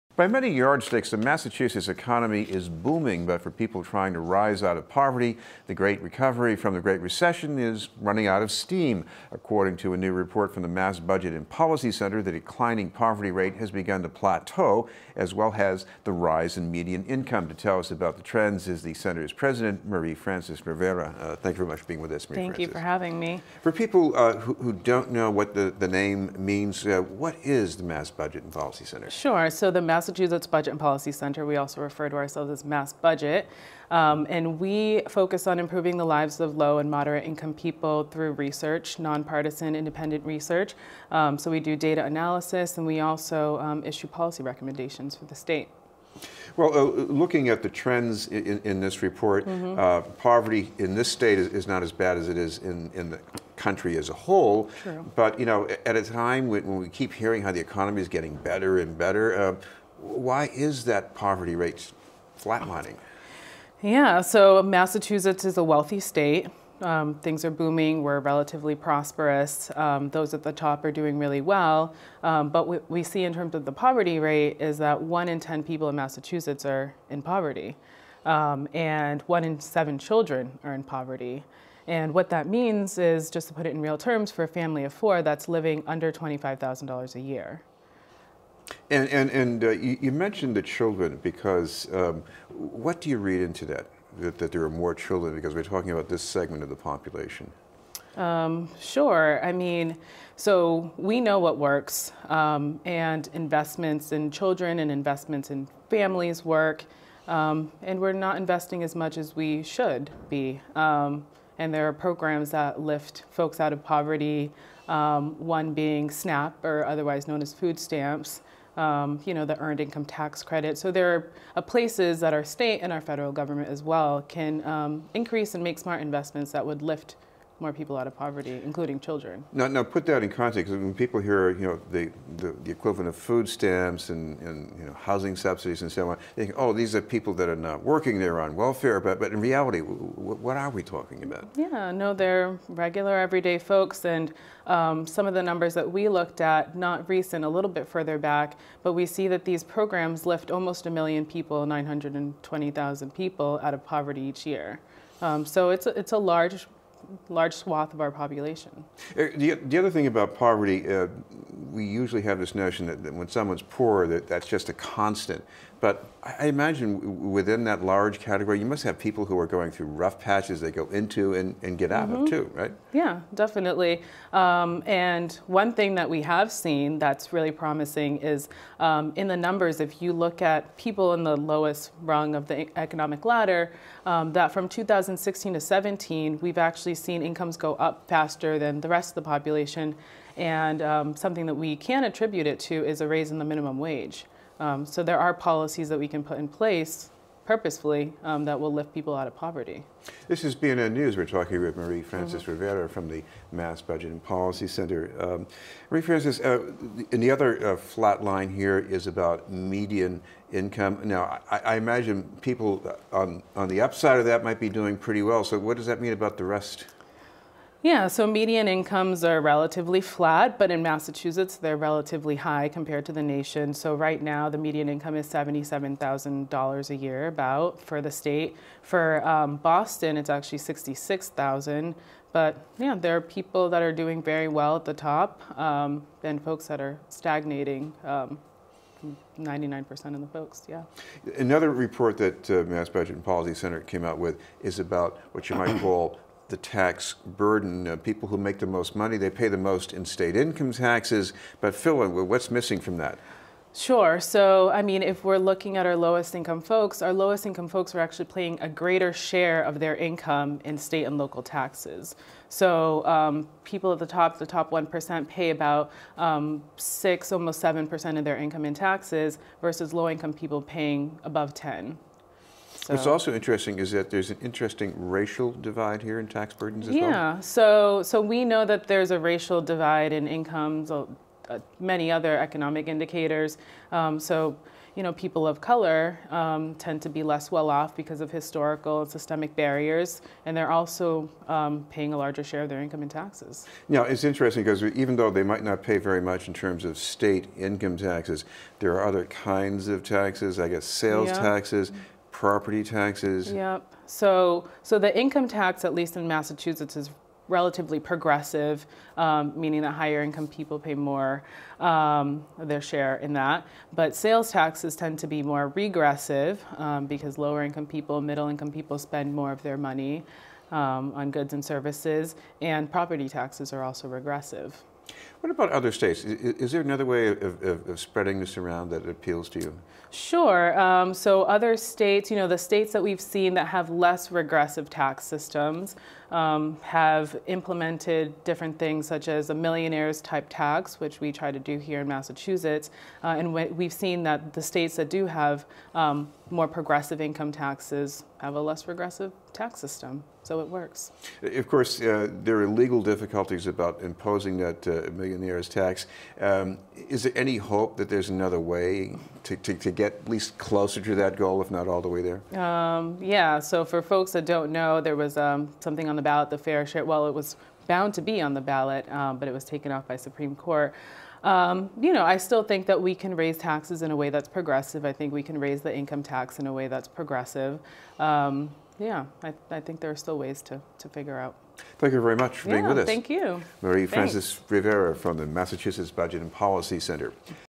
Report: Despite Boom, Poverty Plateaus
The president of the Mass. Budget and Policy Center, Marie Francis Rivera, talks about new reports on stalled progress in reducing the state's poverty rate , and on the racial and economic divide in the state and local tax burden. Interview for BNN News. Aired October 25, 2018.